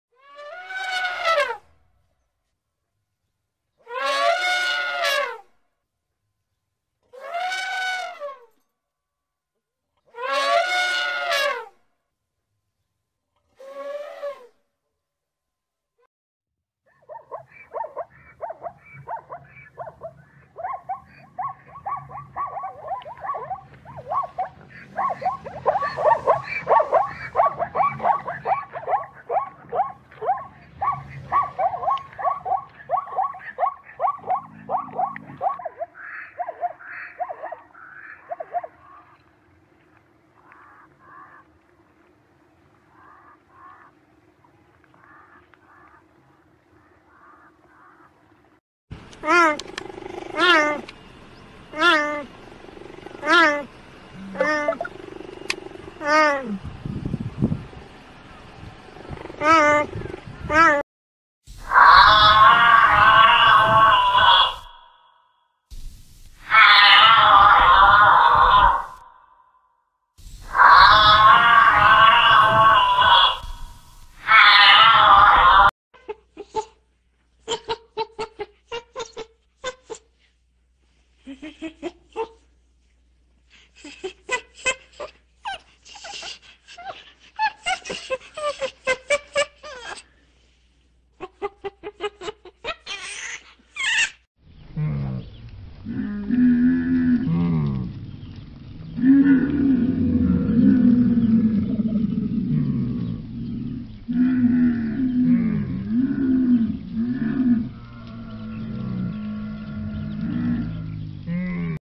Sounds of wild animals. Giraffe, cheetah, elephant, monkey, zebra
Sounds of wild animals for relaxation. Giraffe, cheetah, elephant, monkey, zebra